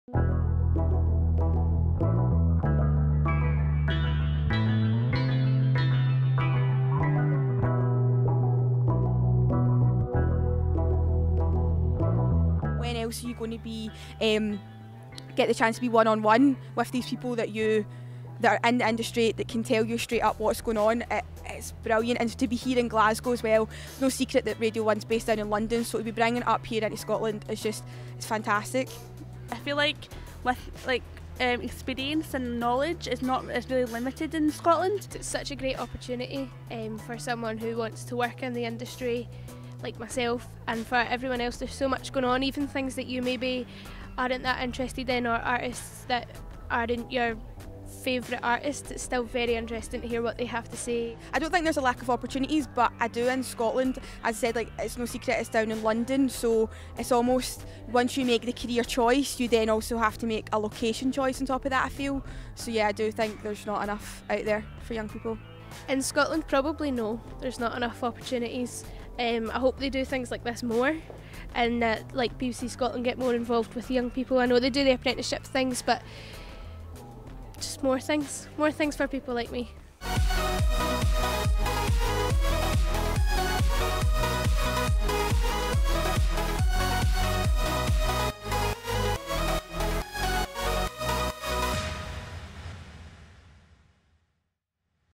GoThinkBig is coming to Glasgow!
The GoThinkBig Sessions are coming to Glasgow in June, so we decided to pop along to the Radio 1 Academy 'Take It On' event at Glasgow University to see what young people think of the employment scene in Scotland at the moment.